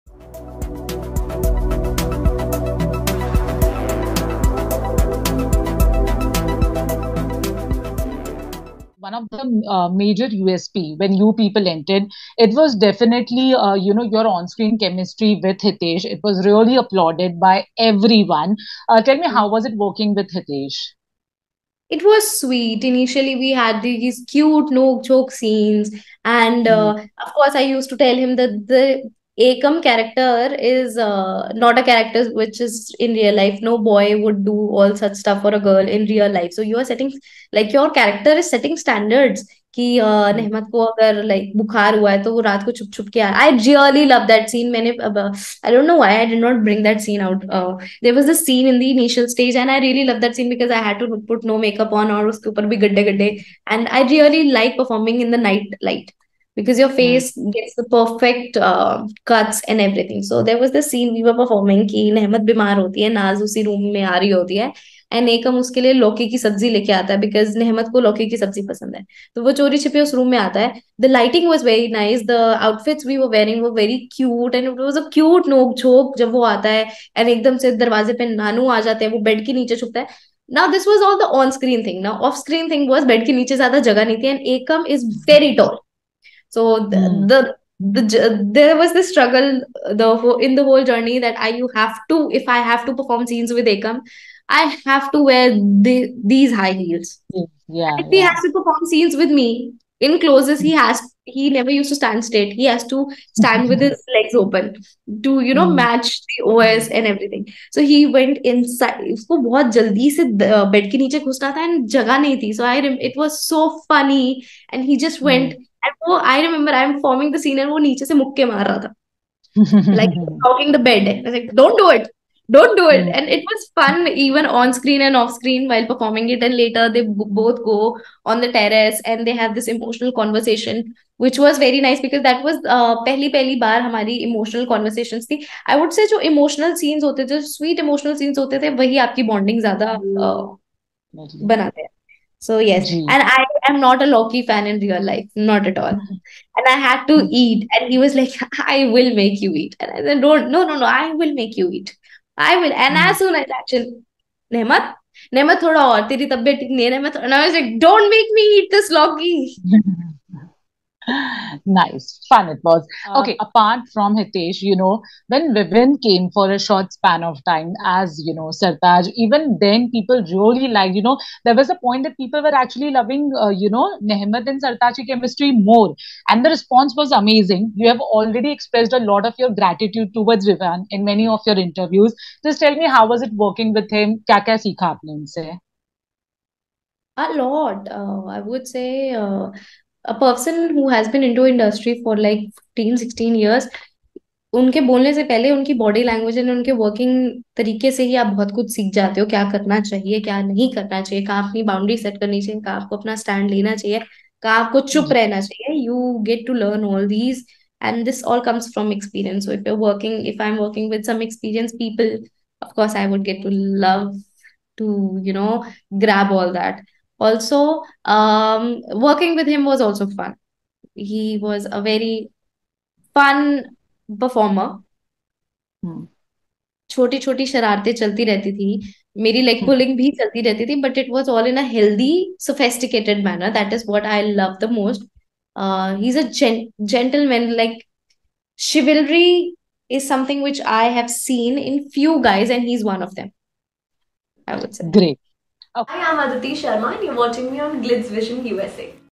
EXCLUSIVE! Udaariyaan FAME Twinkle Arora ON Co-Stars Hitesh Bharadwaj, Vivian Dsena & Bond With Them
Be It NehKam OR NehTaj - there is no denying the fact that they both were splendid and the audience loved them very much. In this candid conversation with @GlitzVisionUSA Twinkl Arora who played Nehmat in Udaariyaan opens up about her bond with Hitesh Bhardwaj (Ekam) and Vivian Dsena (Sartaj) & more.

Interviewer: Kinnari Singh
Video Edited By: VR